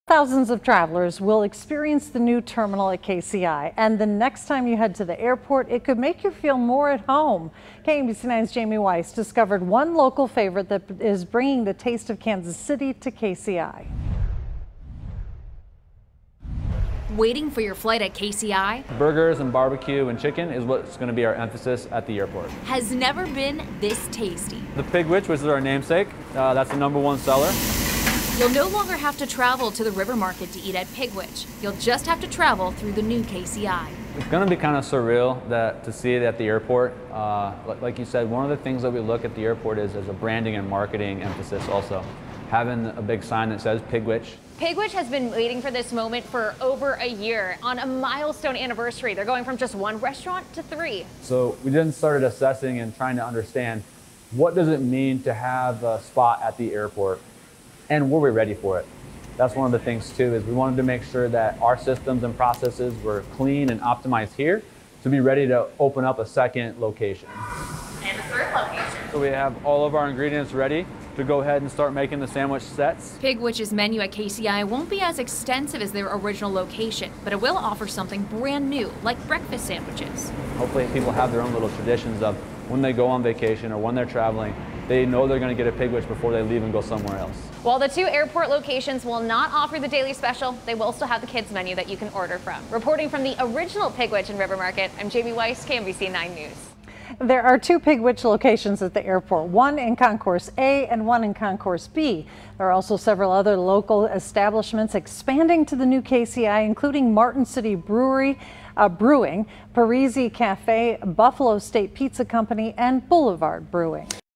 Local eatery Pigwich expands at new Kansas City International Airport
Aired: February 27, 2023 6 PM
Copyright: Hearst Television, KMBC 9